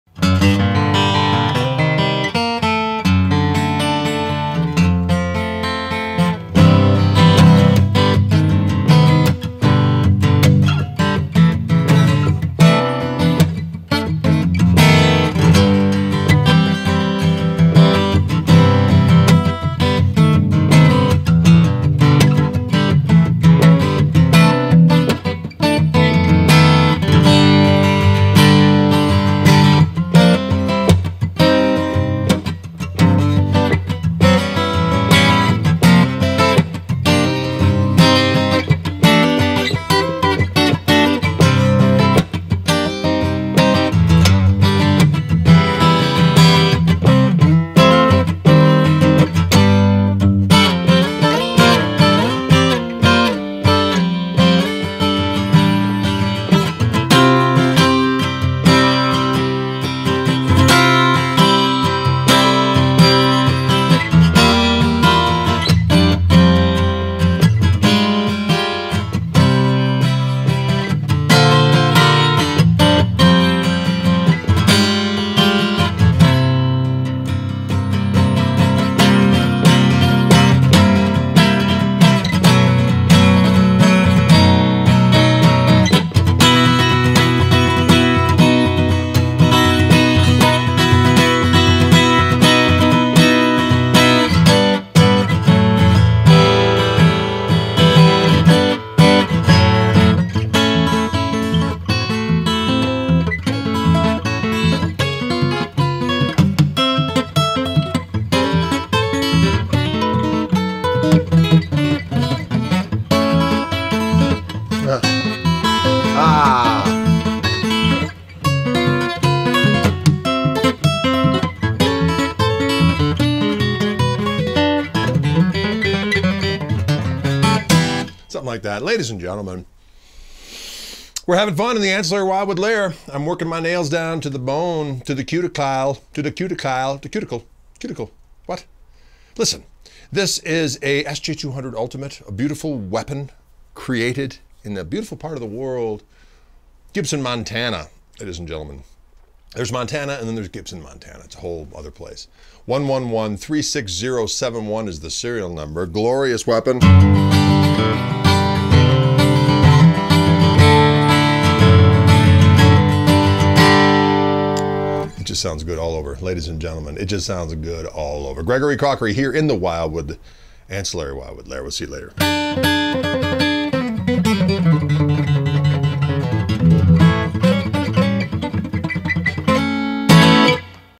Gibson SJ-200 Ultimate • SN: 11136071
The SJ-200 Ultimate delivers the look, feel and sound of our original Super Jumbo as it was at the pinnacle of the acoustic age prior to amplification. Gibson Acoustics master luthiers in Bozeman, Montana, have created a nearly-identical instrument in structure and components as the most popular and celebrated version. Including a Thermally Aged Adirondack red spruce top, hot hide glue top bracing for better tonal response and aged bone nut and saddle. The SJ-200 Ultimate features a Thermally Aged top which captures the deep, well-balanced seasoned tone of an old vintage King of the Flat-Tops SJ-200. Limited to only 40 hand built instruments proudly made in Bozeman, Montana.

Model Limited Edition SJ-200 Ultimate
Limited Series Limited to 40 and Exclusive to Wildwood in N. America
Finish Color Vintage Sunburst
Bracing Traditional scalloped X hot hide glue bracing
Top Wood Thermally Aged Adirondack Red Spruce
Back & Sides Wood Flamed Maple
Neck Wood Flamed Maple
Neck Joint Compound Dovetail secured with Hide Glue at the 14th fret
Fingerboard Rosewood
Fingerboard Radius 12"
Fingerboard Inlays Mother-of-Pearl Graduated Crown
Nut Material Aged Bone
Headstock Crown Peghead Logo
Binding Multi-Ply on Top and Back
Bridge Traditional Rosewood Moustache with two-bar mother-of-pearl inlay
Saddle Aged Bone
Tuners Vintage-style Tuners
Pickguard Tortoise Hand-engraved with the J-200’s traditional Floral and Vine Design
Case Brown Hardshell Case